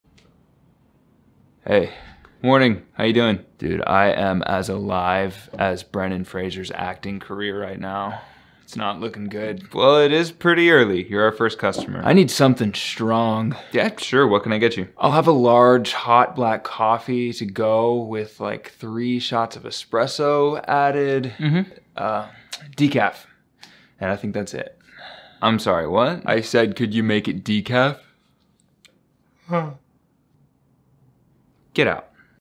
Buying a Decaf Coffee In The Morning
This video is inspired by MANY true events. It's basically a true crime episode. Who orders decaf coffee at 6 in the morning???

IMDb: IMDb.me/JosiahSchneider